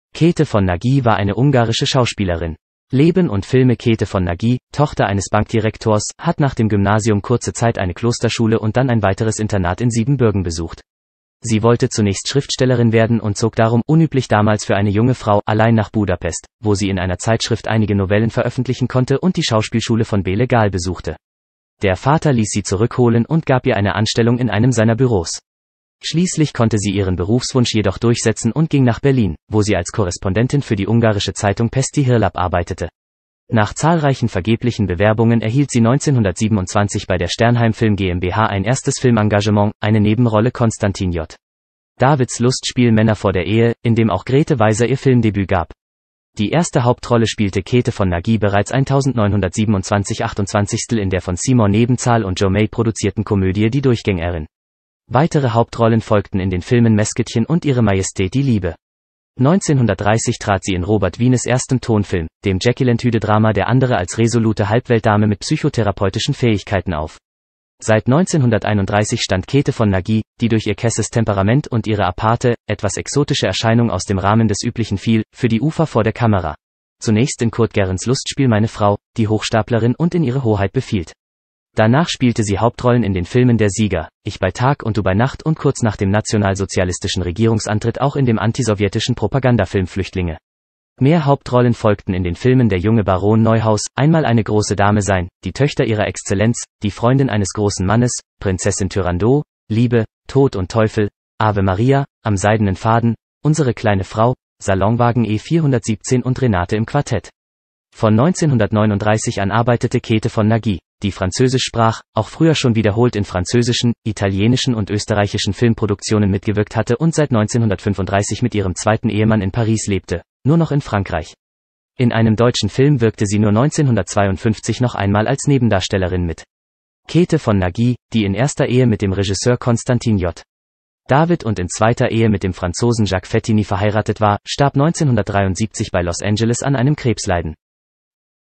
Käthe von Nagy Biografie - Deutsche Schauspieler
Käthe von Nagy  war eine ungarische Schauspielerin.
Leben und Filme
Käthe von Nagy, Tochter eines Bankdirektors, hat nach dem Gymnasium kurze Zeit eine Klosterschule  und dann ein weiteres Internat in Siebenbürgen besucht. Sie wollte zunächst Schriftstellerin werden und zog darum – unüblich damals für eine junge Frau – allein nach Budapest, wo sie in einer Zeitschrift einige Novellen veröffentlichen konnte und die Schauspielschule von Béla Gáal besuchte. Der Vater ließ sie zurückholen und gab ihr eine Anstellung in einem seiner Büros. Schließlich konnte sie ihren Berufswunsch jedoch durchsetzen und ging nach Berlin, wo sie als Korrespondentin für die ungarische Zeitung Pesti Hírlap arbeitete. Nach zahlreichen vergeblichen Bewerbungen erhielt sie 1927 bei der Sternheim-Film GmbH ein erstes Filmengagement, eine Nebenrolle Constantin J. Davids Lustspiel Männer vor der Ehe, in dem auch Grethe Weiser ihr Filmdebüt gab. Die erste Hauptrolle spielte Käthe von Nagy bereits 1927/28 in der von Seymour Nebenzahl und Joe May produzierten Komödie Die Durchgängerin. Weitere Hauptrollen folgten in den Filmen Mascottchen  und Ihre Majestät die Liebe . 1930 trat sie in Robert Wienes erstem Tonfilm, dem Jekyll-and-Hyde-Drama Der Andere als resolute Halbweltdame mit psychotherapeutischen Fähigkeiten auf.